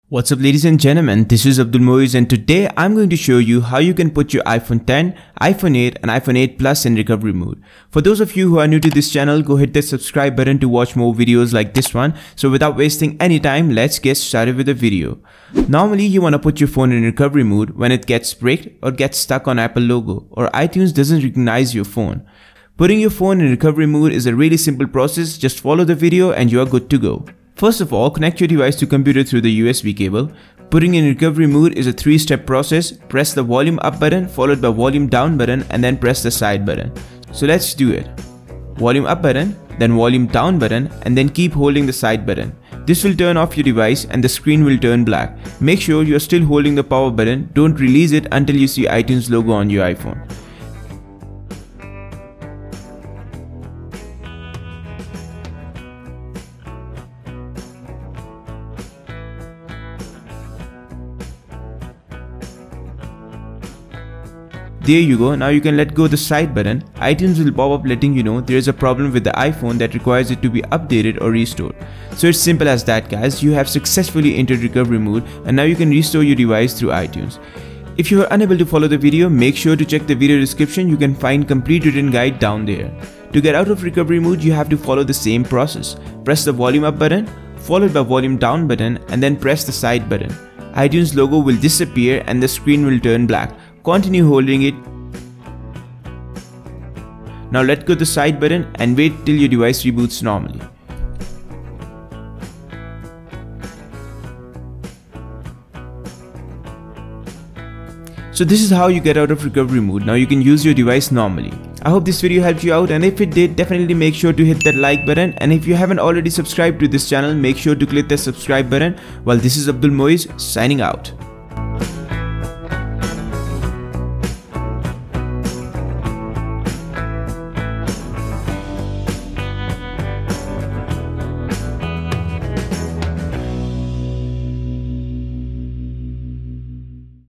How to Put iPhone X / XR / XS / iPhone 8 / 8 Plus in Recovery mode | Restore Mode Tutorial
EASIEST TUTORIAL: iPhone XR/XS /iPhone 8 / iPhone X: how to enter Recovery mode and do Recovery Mode Restore.

How to Enter DFU mode on iPhone X , iPhone 8 & iPhone 8 Plus:

- Abdul Moiz Farooq